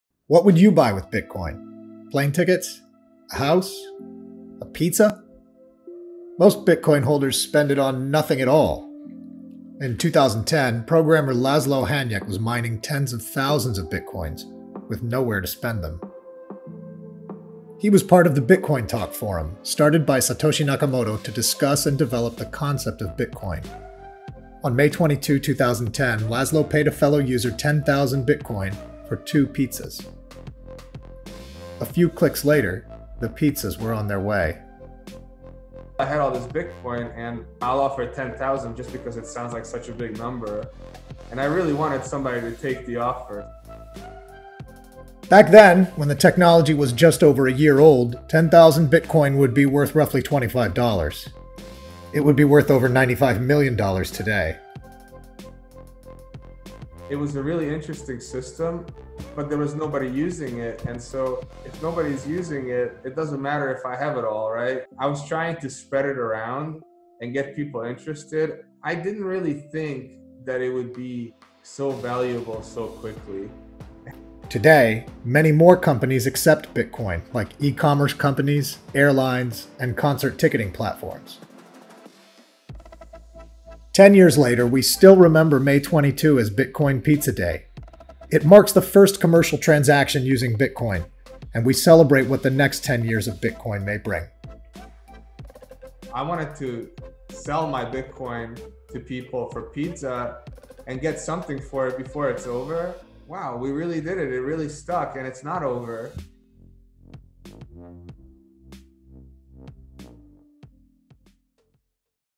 Bitcoin Pizza Day: 10th Anniversary
Ten years ago, a developer named Laszlo Hanyecz bought two pizzas in an online forum for 10,000 BTC. What was worth $25 in 2010 is worth some $95 million today.
That day, May 22, is now known as Bitcoin Pizza Day. We mark the 10th anniversary by chatting with Laszlo Hanyecz about this first bitcoin commercial transaction.

Happy Bitcoin Pizza Day!

CoinDesk Podcast Network: spotify:show:2jyIhkvAQhed8AWDZiPpSP
Markets Daily Crypto Roundup: spotify:show:7sDXM8BlxsUqzL2IqmLqwE